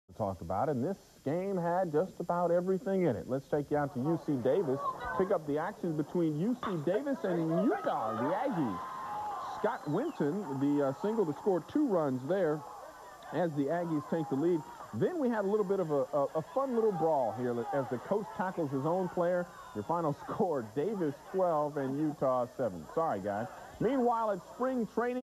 UC Davis vs. Utah baseball game highlights February 1995
Sacramento news highlights of UC Davis vs. Utah baseball game in Davis, CA (February 1995). UC Davis won the game 12-7.